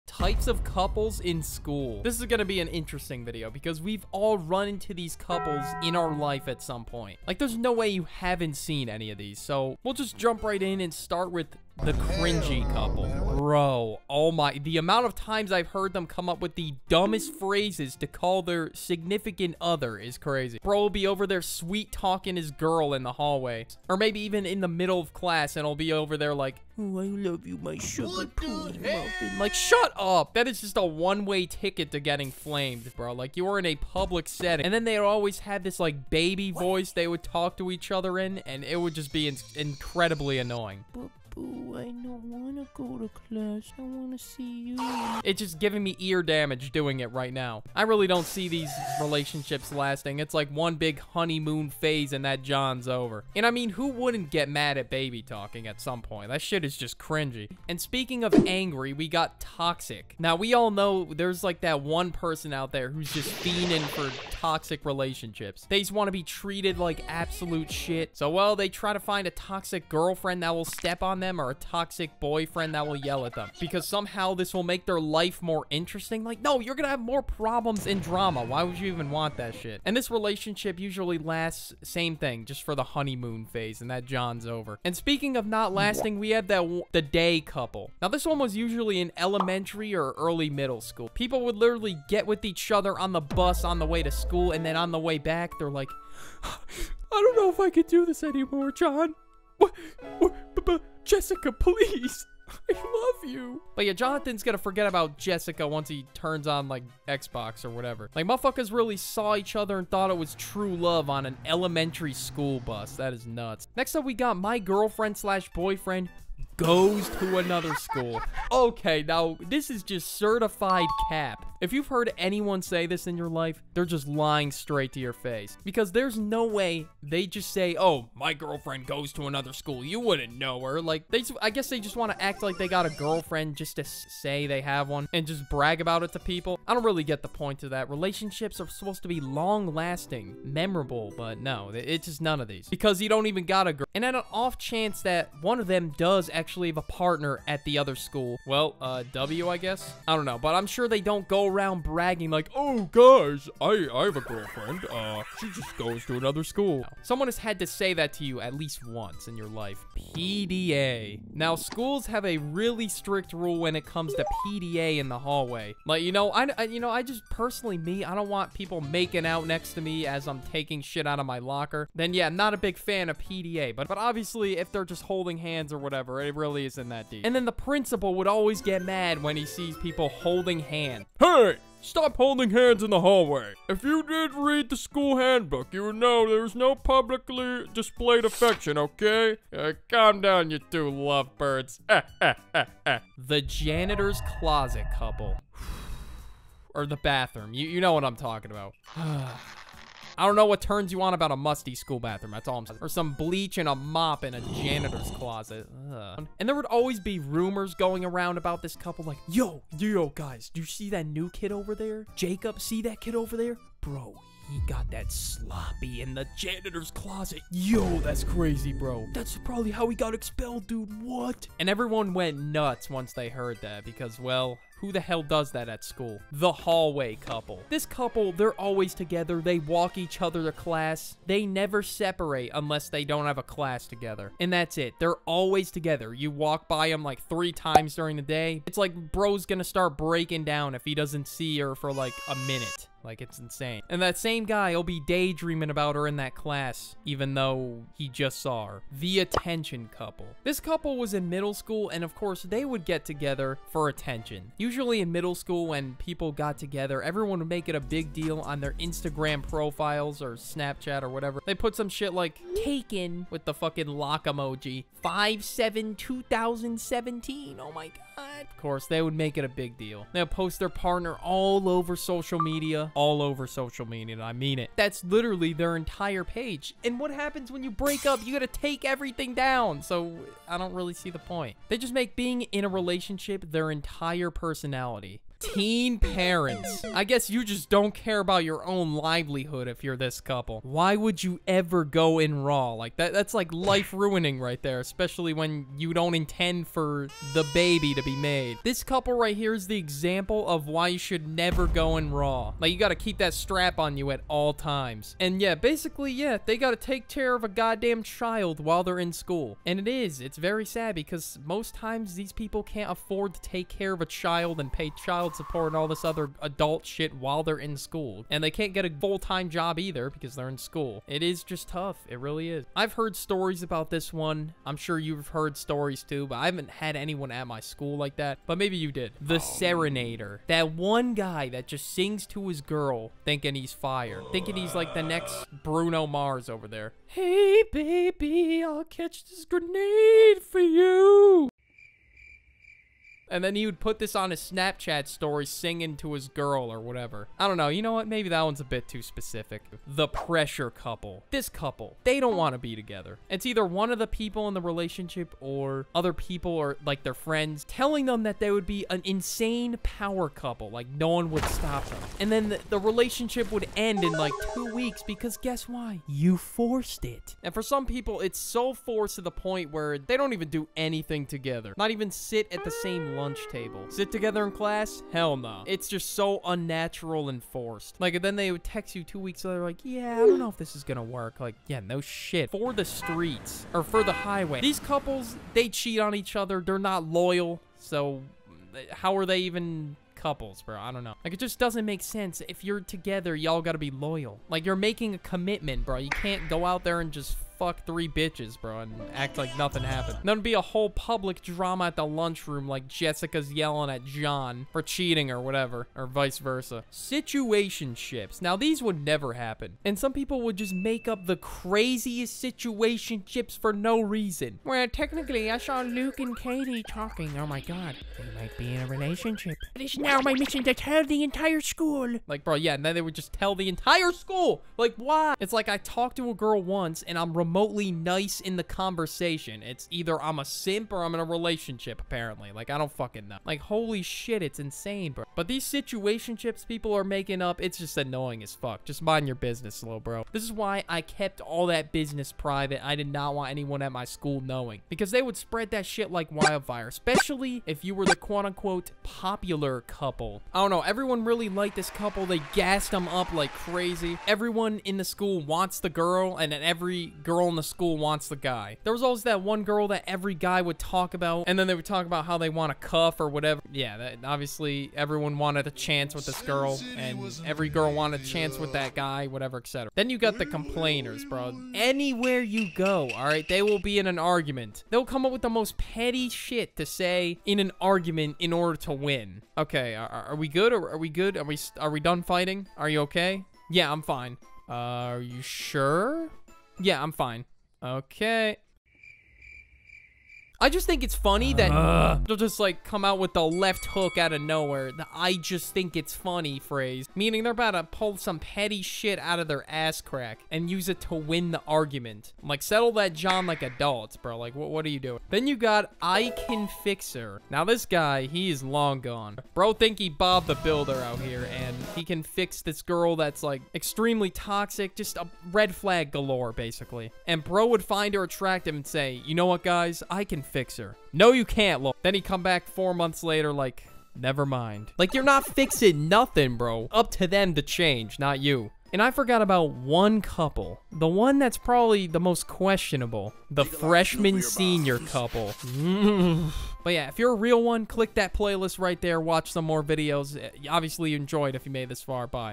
Types Of Couples In School...
Types Of Couples In School... these are the types of couples in school and there are many types of couples along many different types of kids in high school